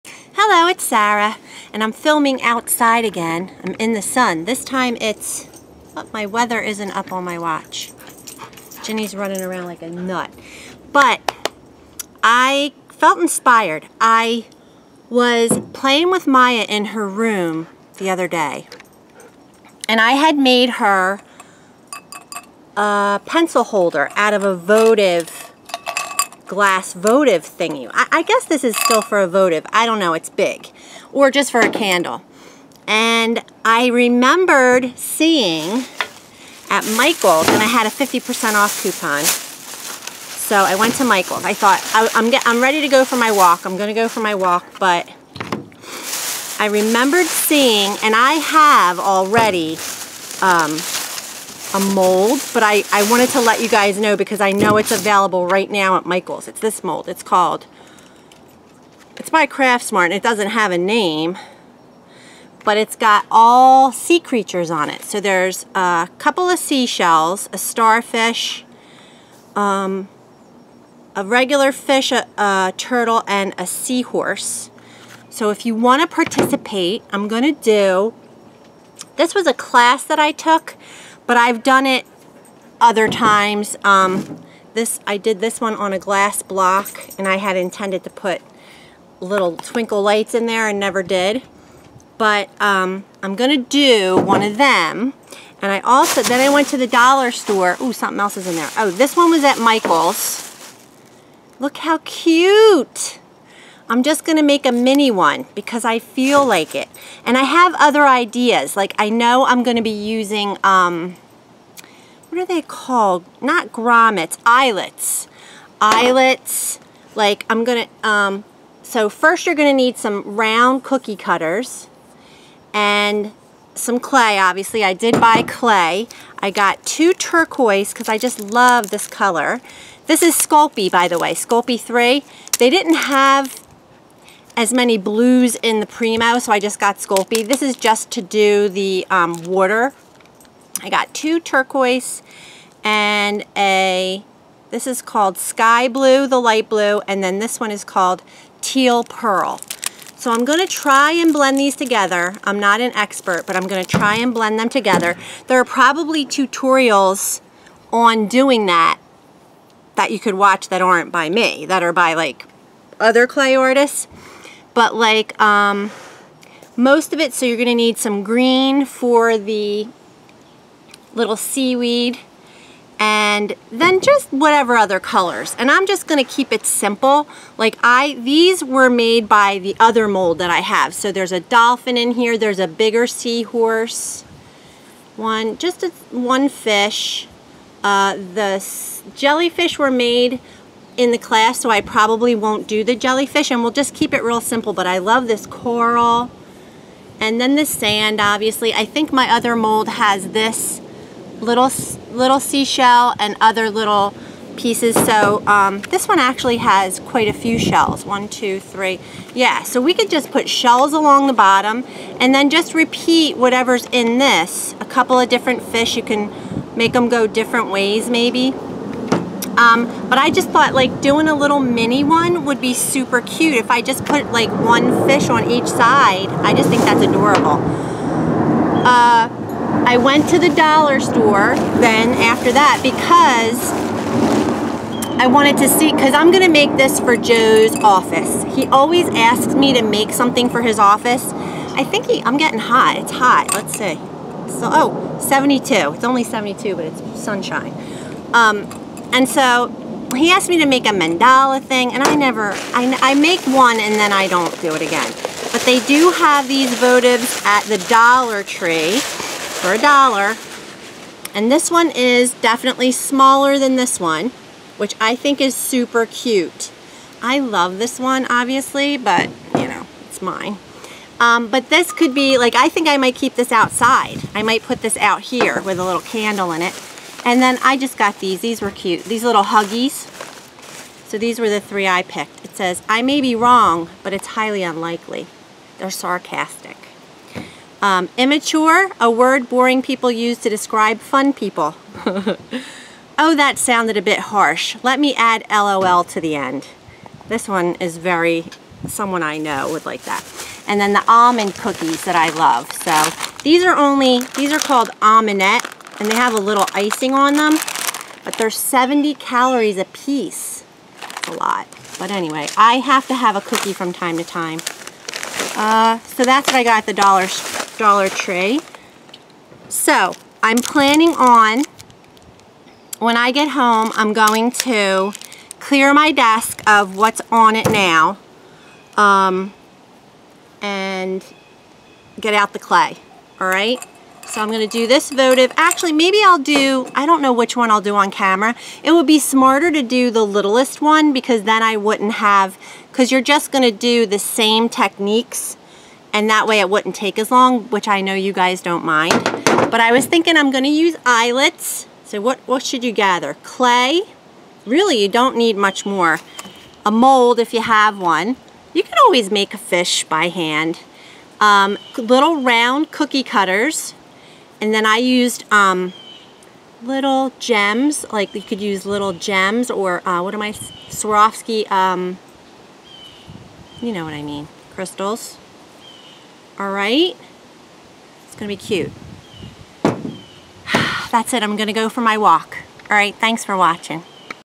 Fish tank votive Prep
I will be doing a tutorial on a fish tank votive with polymer clay. You will need; a glass votive, polymer clay in ocean colors, small round cookie cutters, craft smart sea critter mold from Michaels and a pasta machine if you have one. I'll be back later to create. TFW, Sara